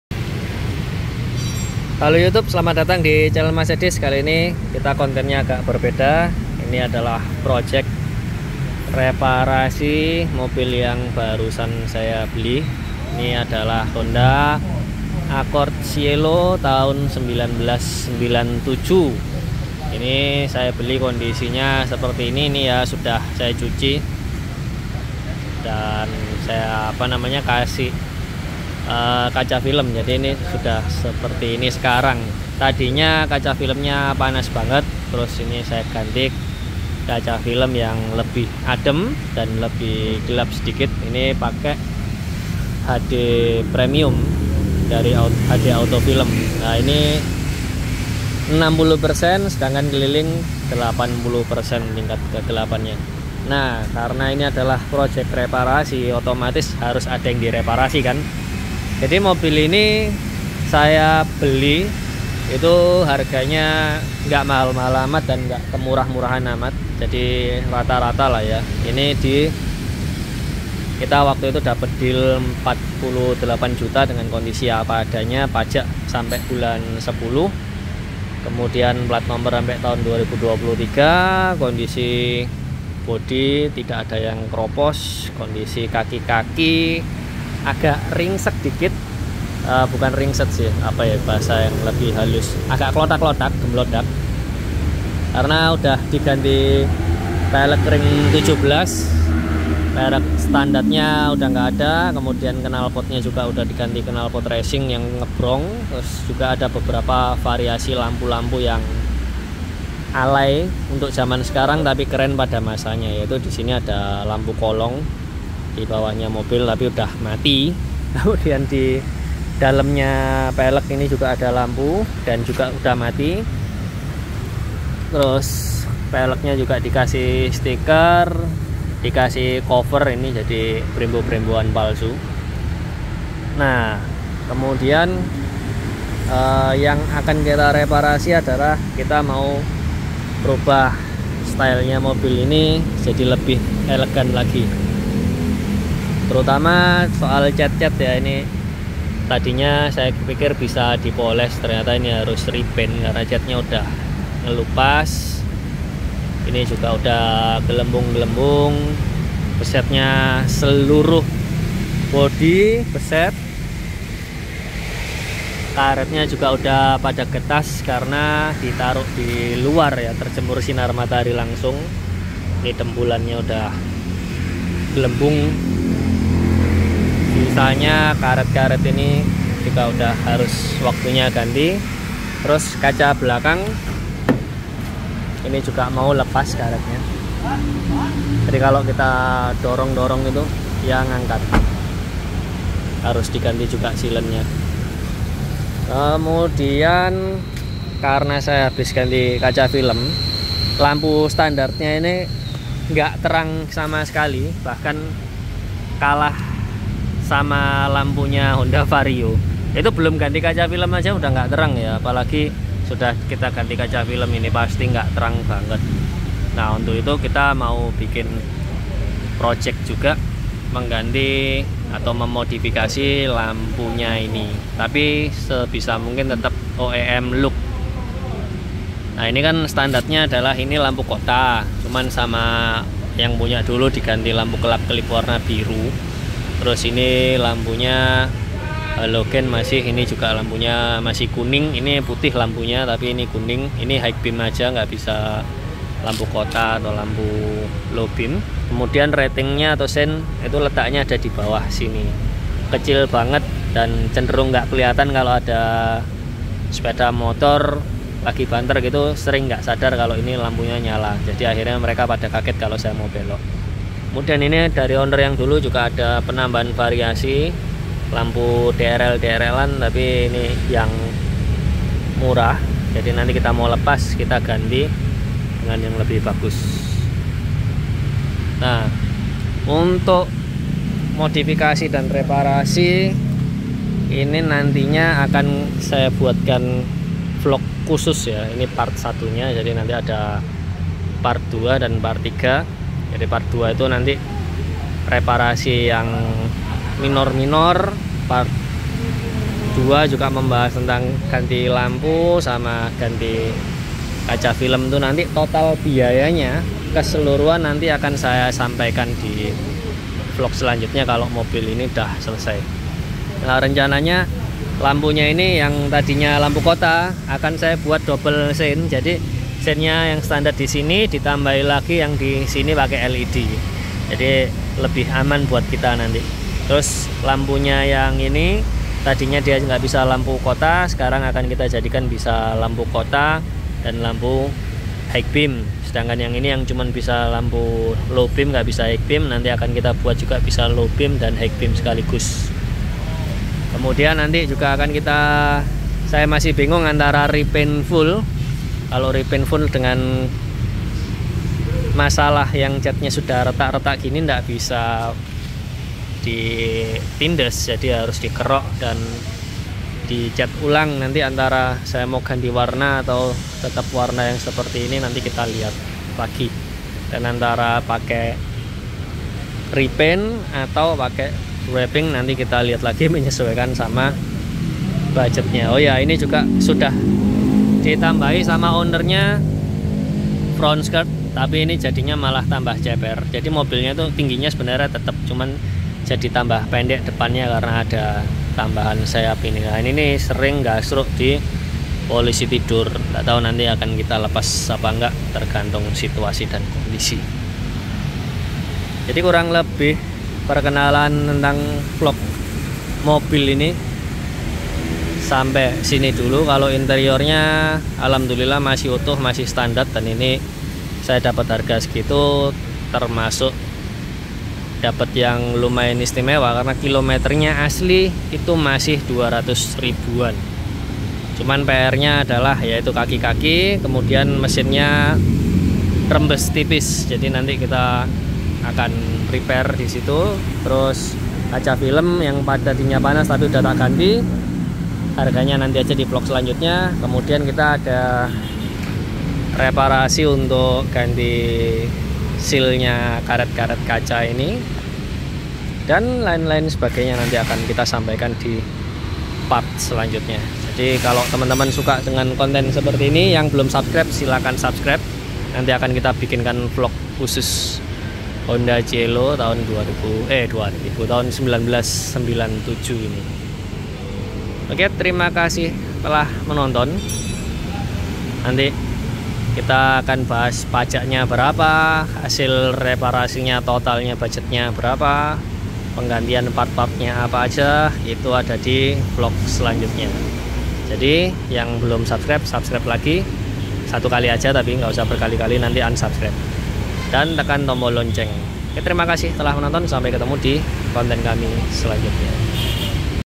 Beli Mobil Tua,Siapa Takut!!! Accord Cielo VTEC 1997 | Jurnal Cielo #1
mobilnya kaum mendang-mending seperti saya

murah tapi harus mewah!